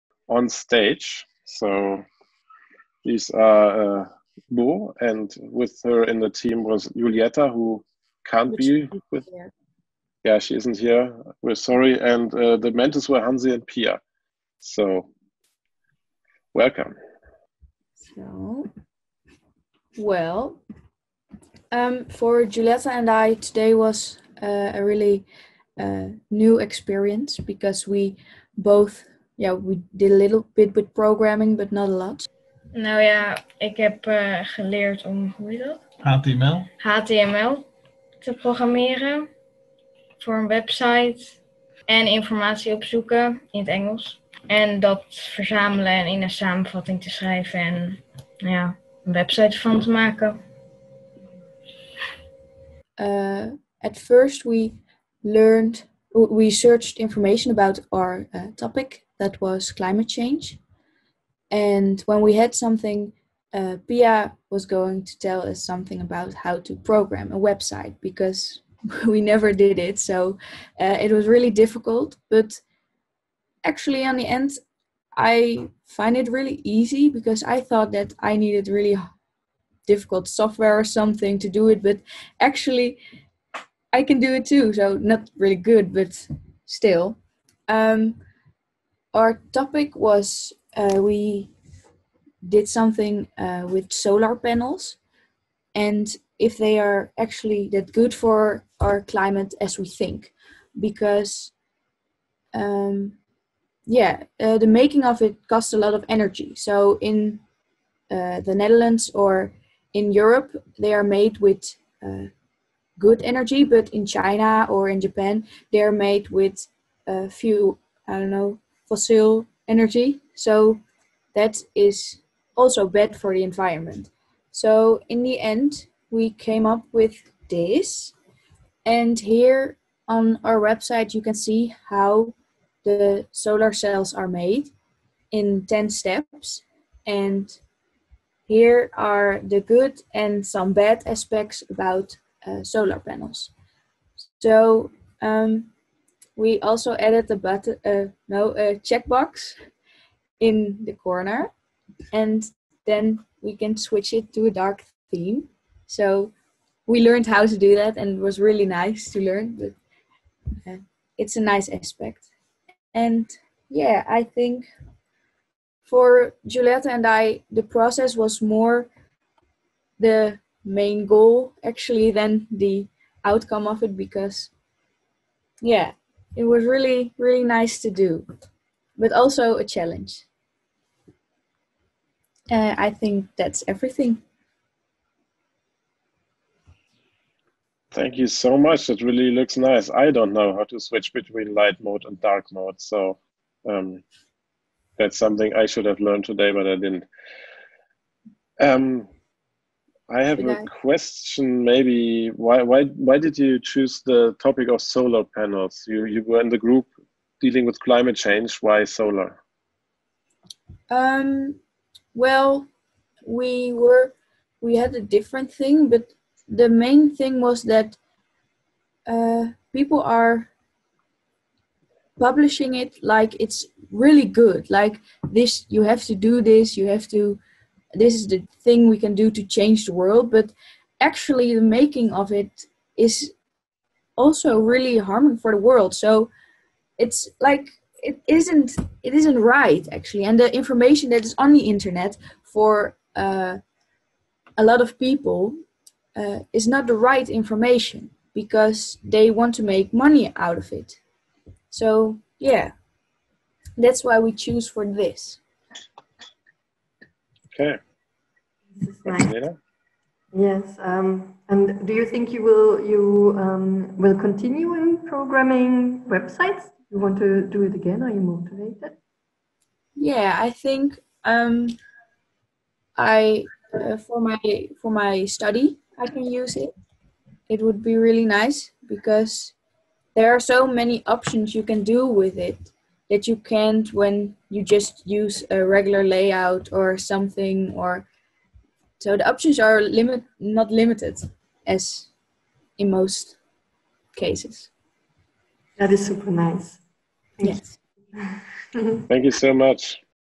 Are solar panels as shiny as they look like? | Jugend hackt Linz 2020
Das Projekt „Are solar panels as shiny as they look like?“ hinterfragt den umweltfreundlichen Ruf von Solaranlagen und informiert über deren Herstellungsprozess. Denn im Gegensatz zu deren ökologischer Energieproduktion ist ihre Herstellung nicht immer naturverträglich oder energiesparsam.

wo?

was?